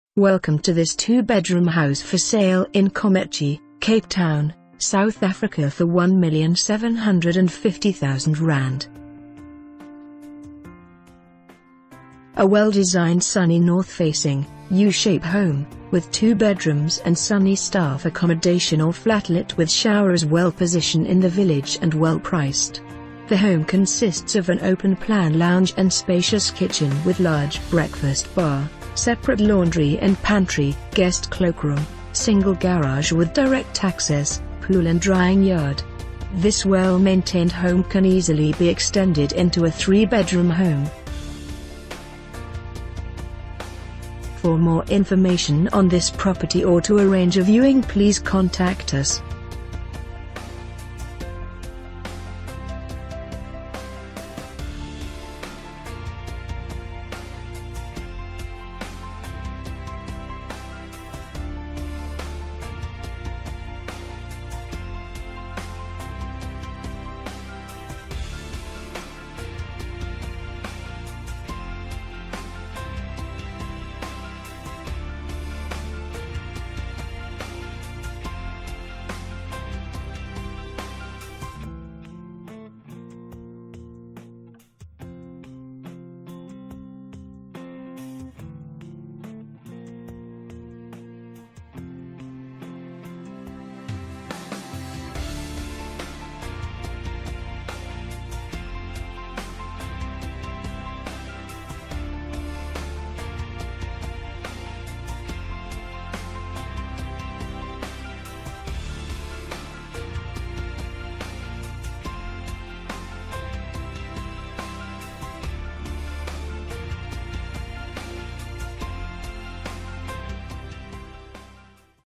2 Bedroom House For Sale in Kommetjie, Cape Town, South Africa for ZAR 1,750,000...
A well designed sunny north facing, u shape home, with 2 bedrooms and sunny staff accommodation or flatlet with shower is well position in the village and well priced. The home consists of an open plan lounge and spacious  kitchen with large breakfast bar, separate laundry and pantry, guest cloakroom, single garage with direc...